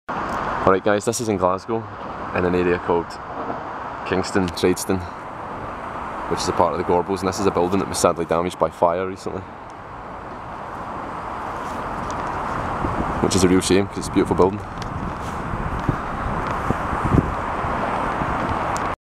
Co-Op Funeral Building Glasgow (Student Tours Scotland STS 130)
The Co-Op Funeral Building in the Glasgow Tradeston/ Kingston region was sadly gutted by fire in 2011. Tried to get a wee video of what remains before it all comes down soon. Shame.